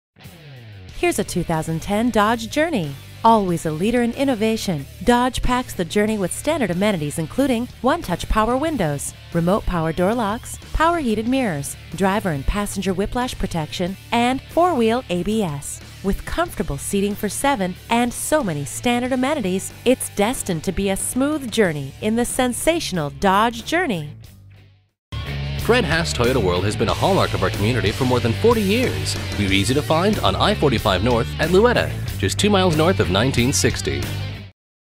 2010 Dodge Journey Spring Houston, TX AT257025T - SOLD
Year: 2010
Make: Dodge
Model: Journey
Trim: SE
Engine: 2.4l 4 Cyl. Sequential-Port F.I.
Transmission: FWD 4-Speed Automatic
Mileage: 96776
Stock #: AT257025T
VIN: 3D4PG4FB3AT257025
You've never felt safer than when you cruise with anti-lock brakes, traction control, dual airbags, and emergency brake assistance in this 2010 Dodge Journey SE. It comes with a 2.4 liter 4 Cylinder engine. With an unbeatable 5-star crash test rating, this SUV puts safety first. According to a review from New Car Test Drive, Each bin can hold up to six soda cans plus ice. Come by today to see this one in person!

Address:
20400 I-45 North